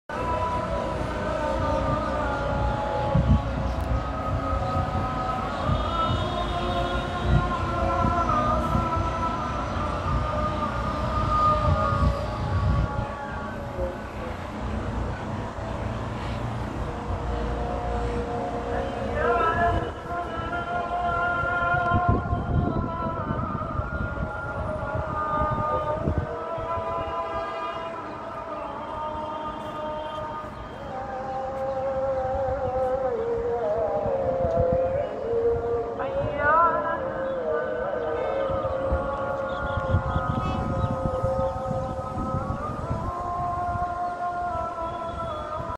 ISRAEL | Nazareth view from roof of Shopping Building
Nazareth in Israel  - an amazing city.

Jesus grew up here 2000 years ago.